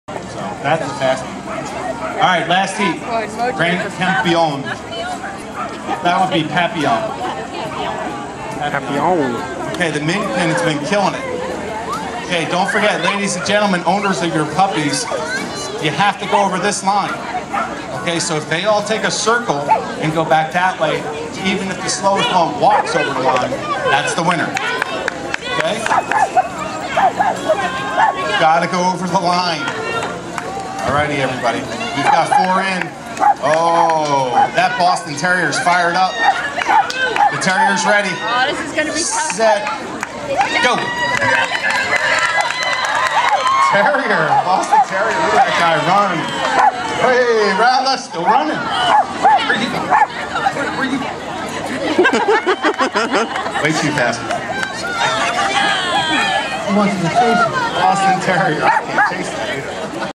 Under 25 lb race Finals
Held at the 10th Annual Your Dog Is Worth It Too Day  at L'Oreal in Cranbury, NJ